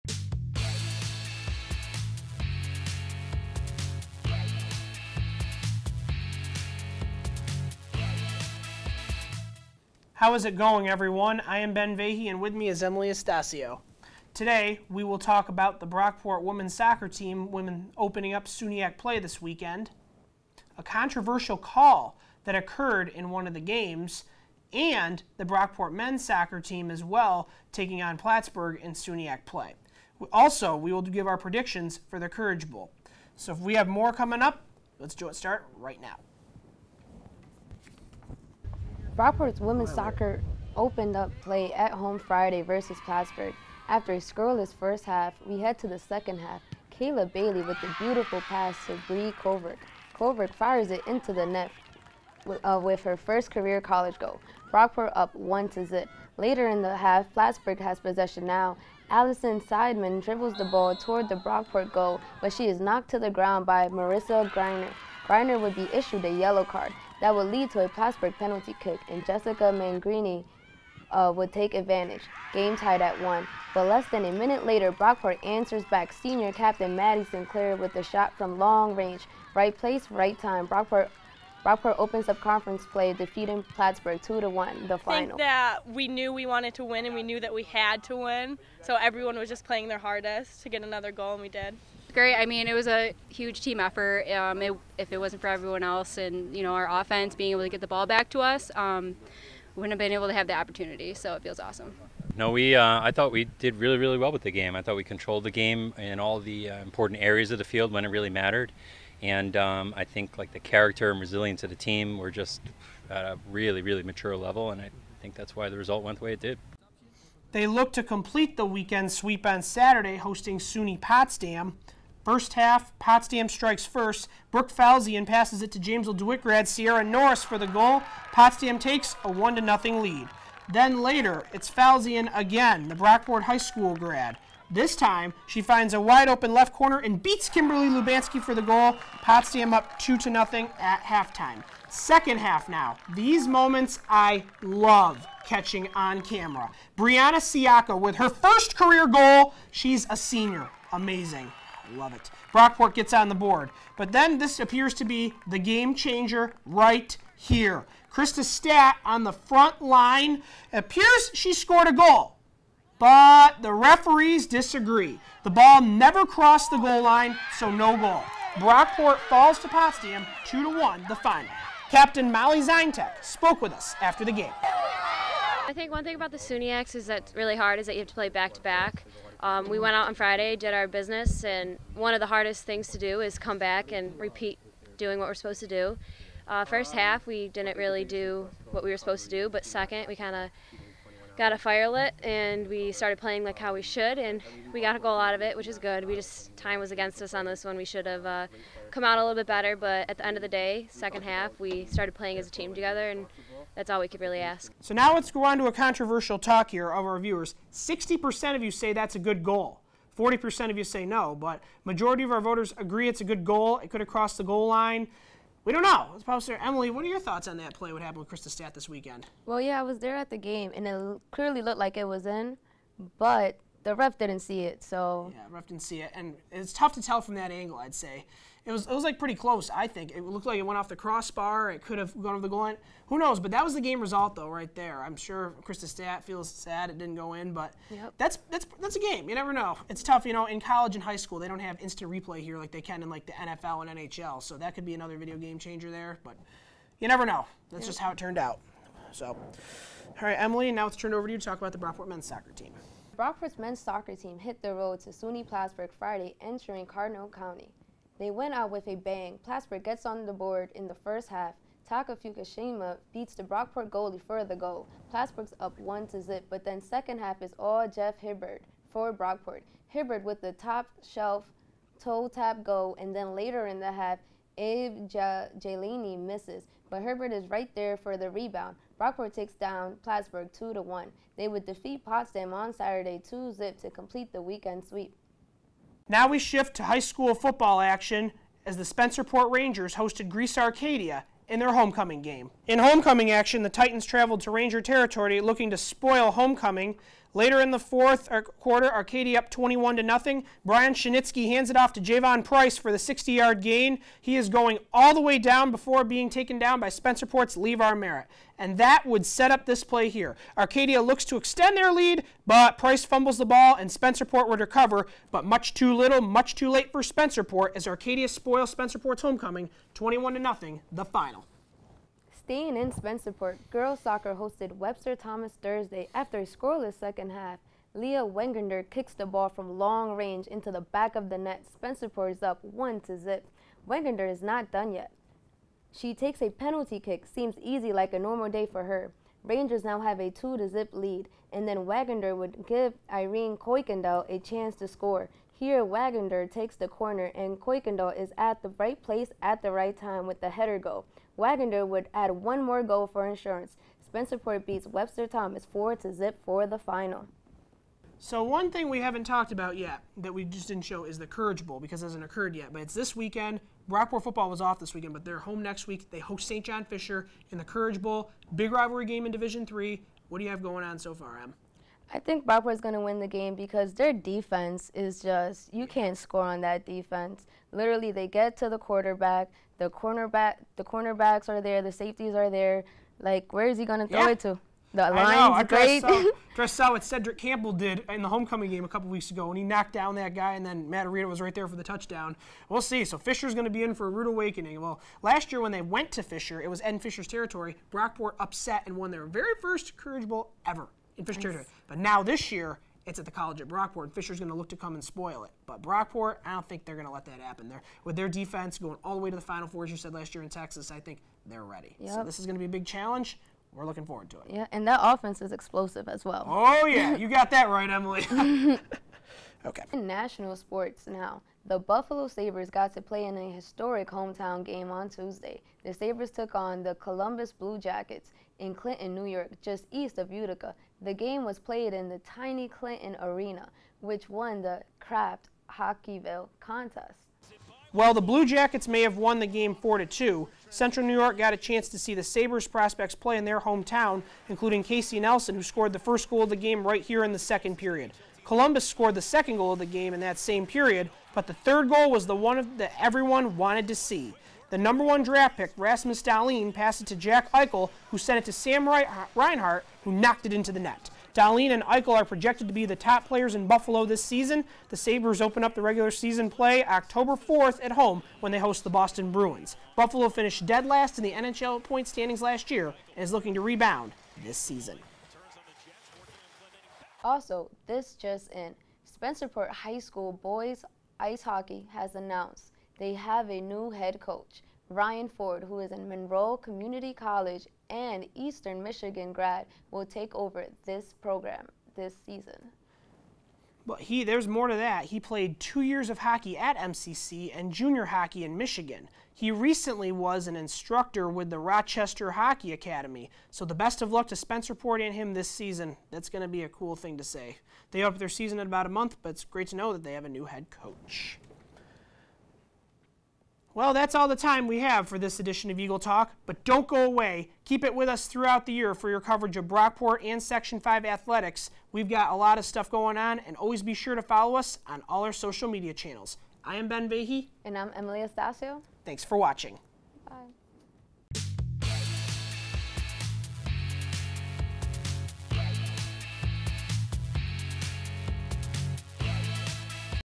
Eagle Talk: Brockport Women's Soccer SUNYAC openers, and Spencerport Action
In this edition of Eagle Talk, anchors Ben Vahey and Emily Astacio recap the Brockport Women's Soccer SUNYAC opening games, talk Courage Bowl predictions, and head to Spencerport for Football and Soccer action.